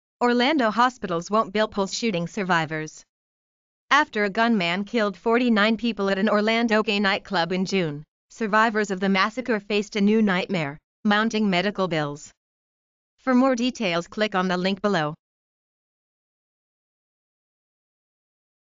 Orlando hospitals won't bill Pulse shooting survivors | By : CNN
After a gunman killed 49 people at an Orlando gay nightclub in June, survivors of the massacre faced a new nightmare: mounting medical bills.